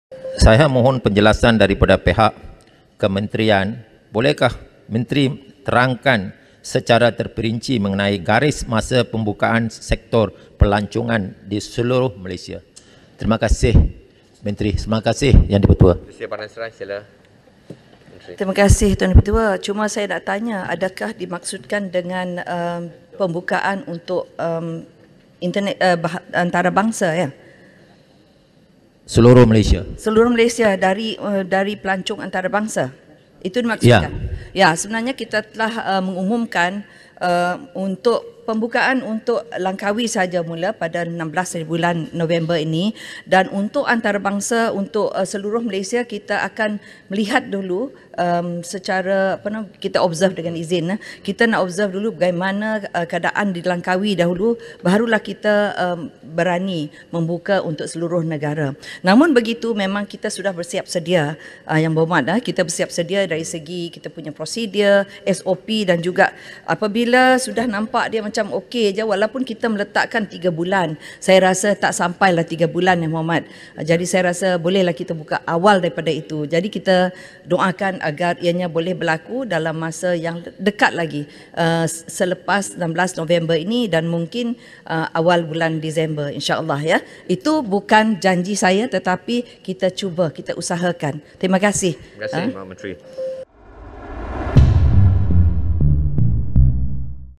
Nancy Shukri: International borders could be reopened by early December
Tourism, Arts and Culture Minister Nancy Shukri said international borders may be reopened nationwide by early December, depending on the situation of the international tourism travel bubble pilot project in Langkawi.

The minister said the situation in Langkawi needs to be observed first before the international borders can be reopened.

“We had placed a three-month (evaluation period for Langkawi). I do not think it will take three months, I think we can reopen earlier than that."